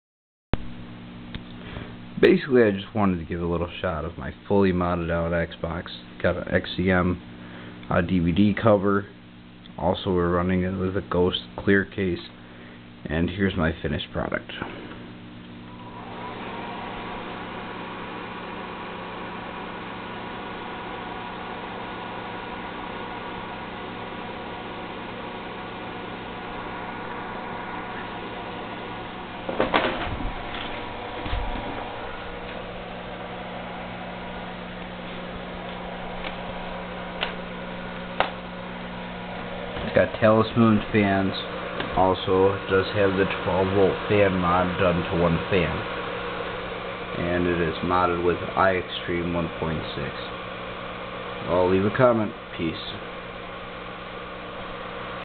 ultimate Jasper xbox 360 fully modded 360
Fully Modded "jasper xbox 360"
Ghost case
12v fan mod on one side of talismoon whisper green fan
2 blue led strips in the back of the machine
Talismoon aurora light kit
Xcm benq/philips clear dvd drive cover
just finished putting my machine together at 2:35 am now 2:42 am